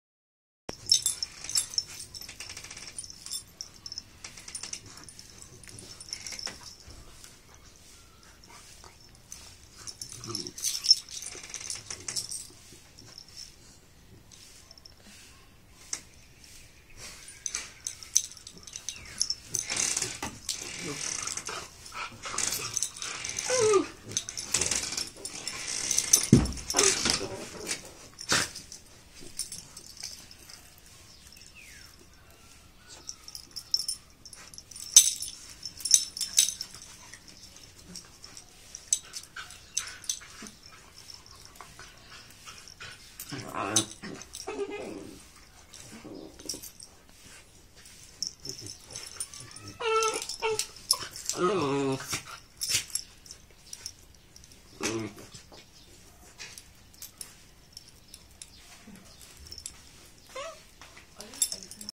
Lupin vs Sabrina
Almost 6:00 in the morning Lupin and Sabrina are playing by biting each other! ;) Bite time before walking outside!